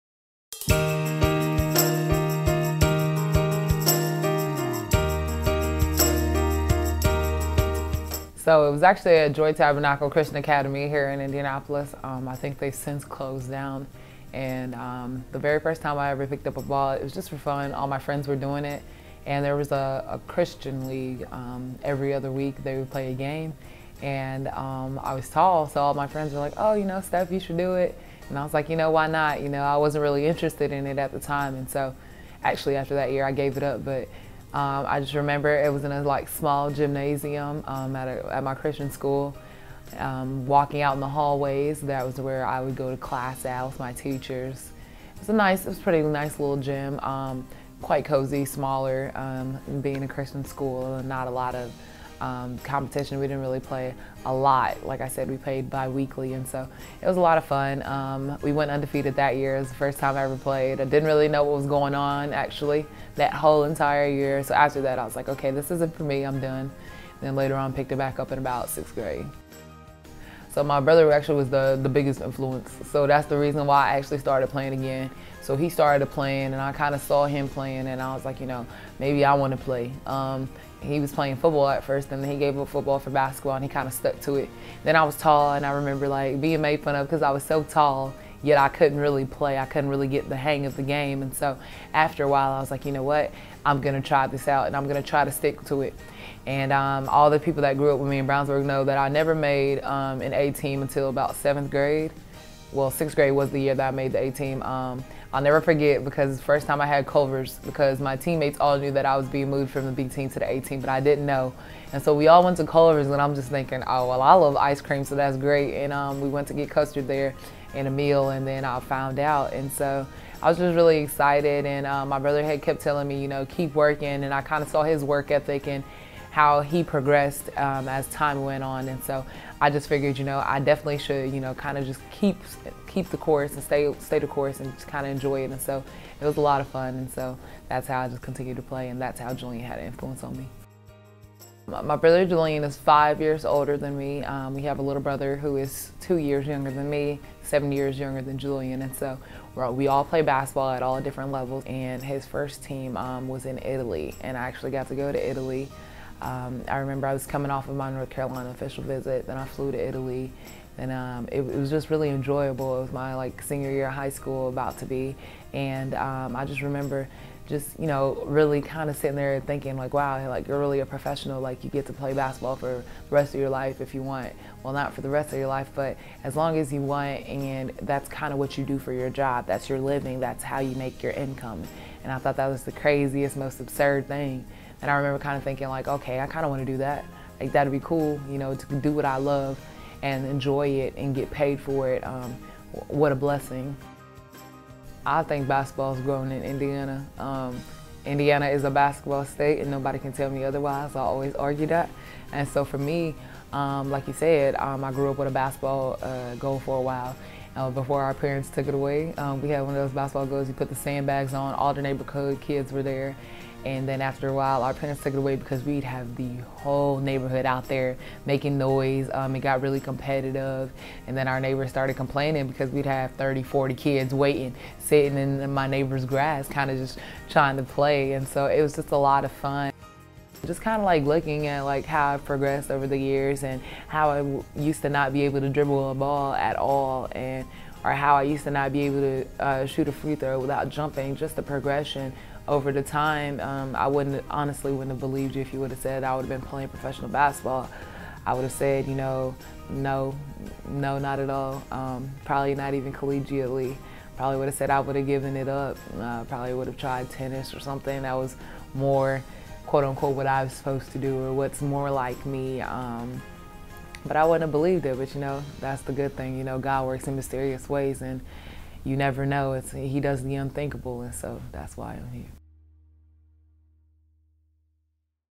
My First Hoop: Stephanie Mavunga
Indiana native Stephanie Mavunga almost gave up on basketball after her first year playing. But after watching her big brother excel in the sport, Mavunga decided to give basketball another chance